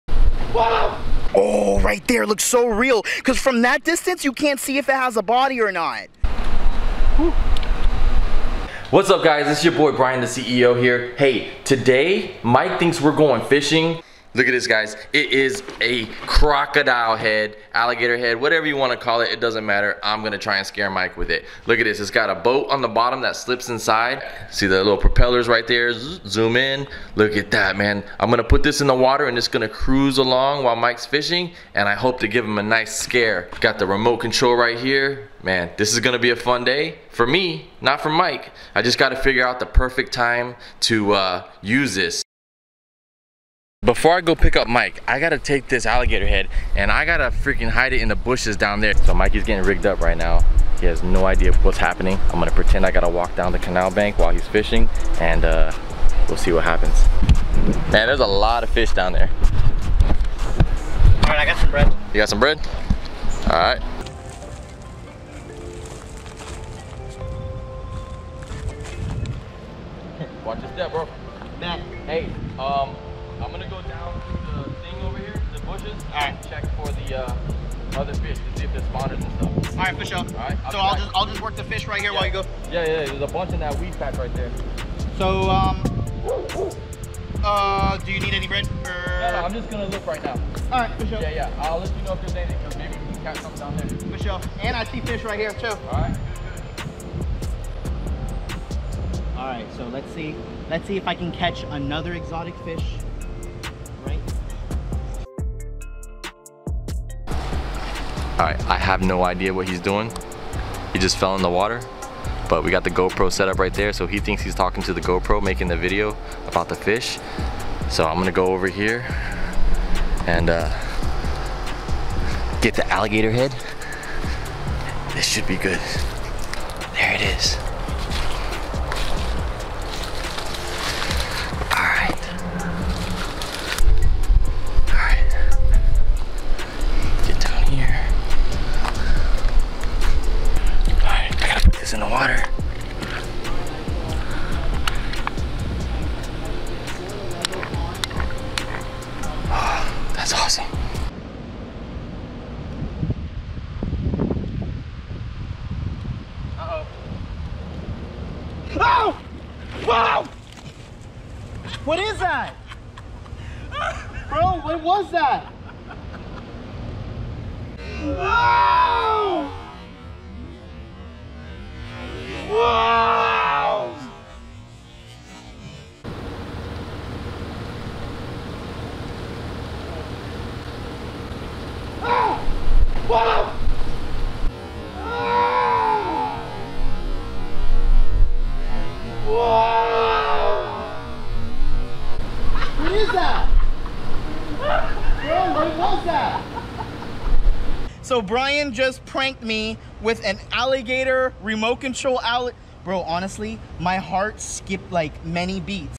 Fake Alligator Prank! Remote Control Alligator Head
I decided to prank Monster Mike with a fake remote controlled alligator head.  It took some planning, but he never saw it coming.  I set him up on our fishing trip.
Snap: thehooksetlife